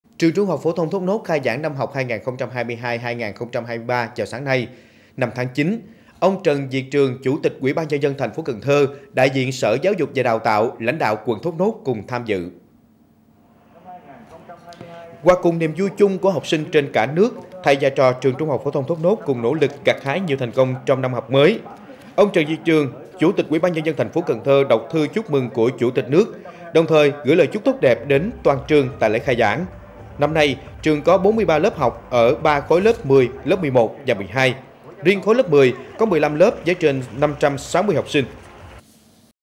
Trường THPT Thốt Nốt khai giảng năm học 2022 - 2023 | Cần Thơ TV
Trường THPT Thốt Nốt khai giảng năm học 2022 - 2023 vào sáng nay (5/9). Ông Trường Việt Trường - Chủ tịch Ủy ban nhân dân thành phố Cần Thơ, đại diện Sở Giáo dục và Đào tạo, lãnh đạo quận Thốt Nốt cùng tham dự.